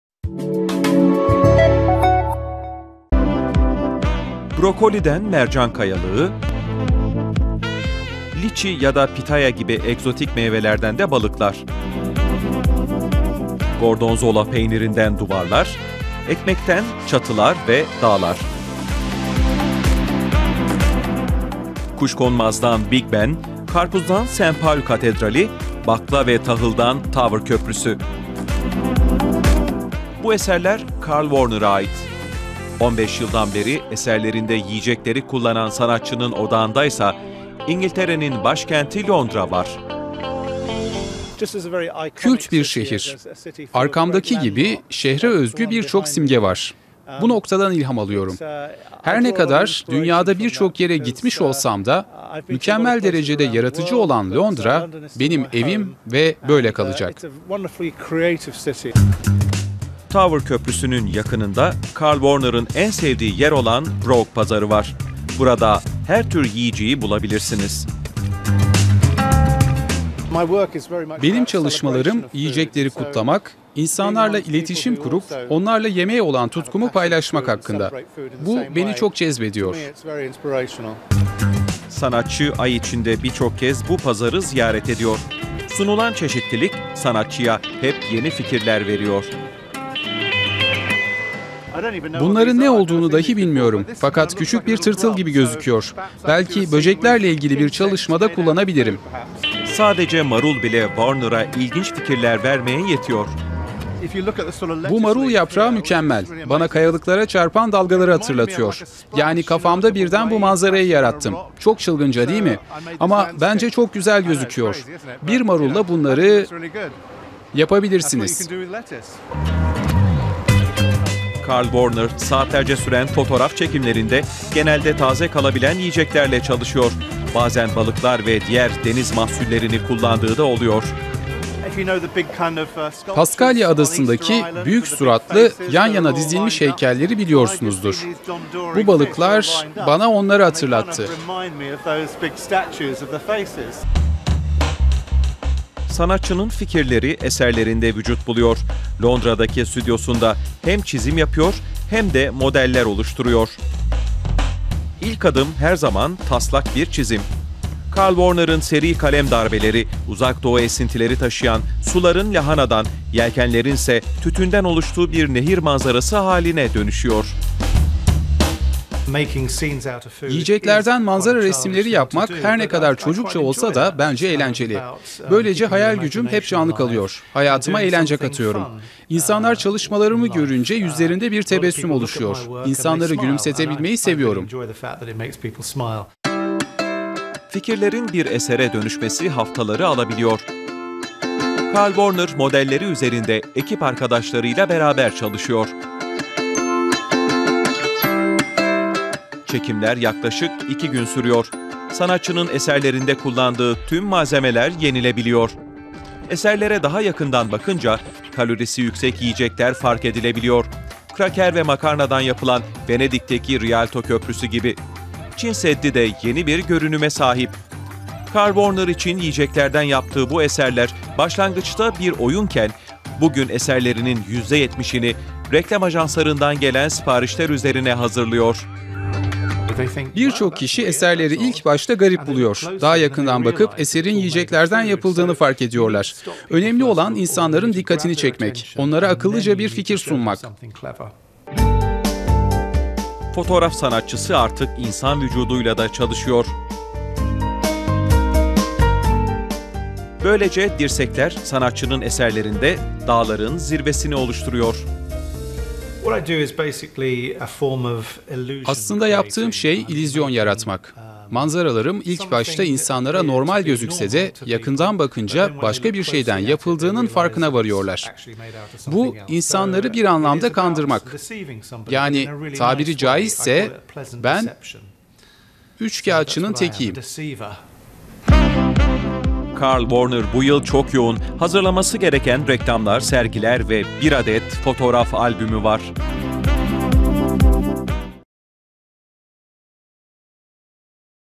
Karın acıktıran sanat - DW Türkçe
Londralı fotoğraf sanatçısı Carl Warner'ın eserleri ilk bakışta sıradan bir manzara gibi gözükse de eserlerinin ayırıcı özelliği kullanılan malzemelerin sadece yiyecekler olması.